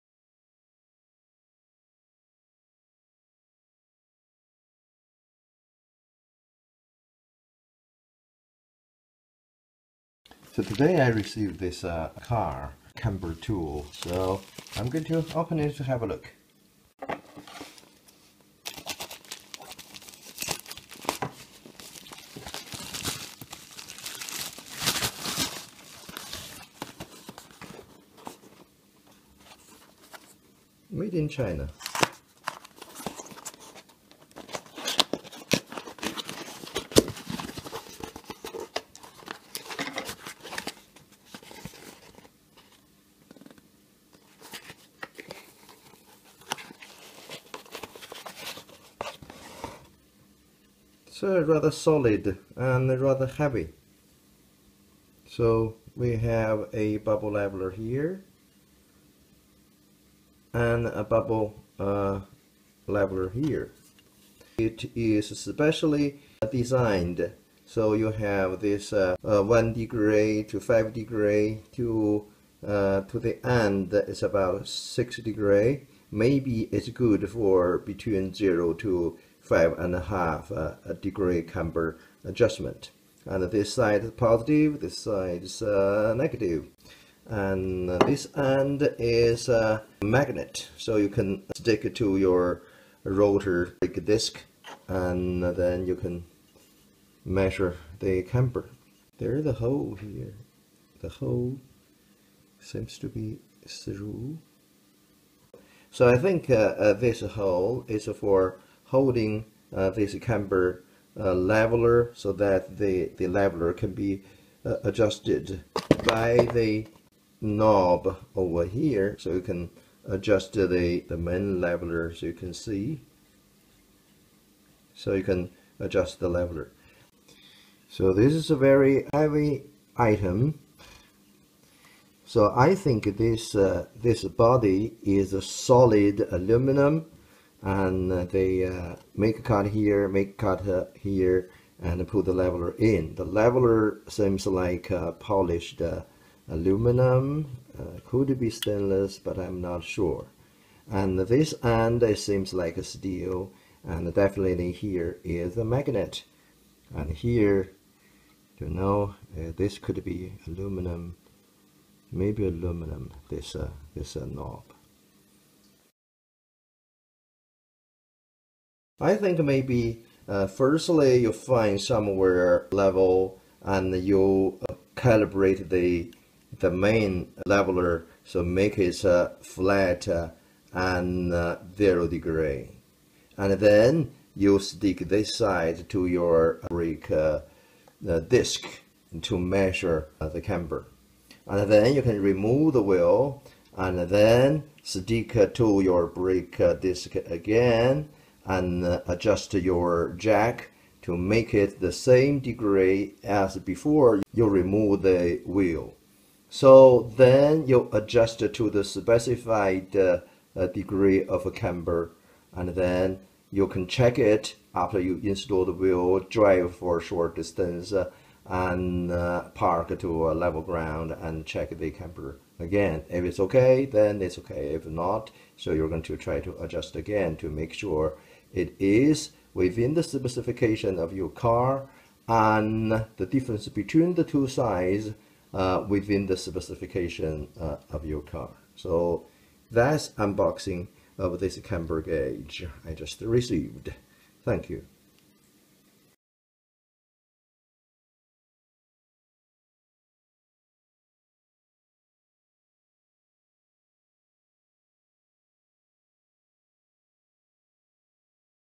Car Suspension Tool: Camber Gauge: Unboxing
Car Suspension Tool: Camber Gauge: Unboxing (#261)

The camber levelling gauge is useful to check and adjust the camber of your car wheels.

1. The Car Suspension Camber Leveller
2. How to Use Camber Leveller

If you like this video, please click the thumbs-up icon.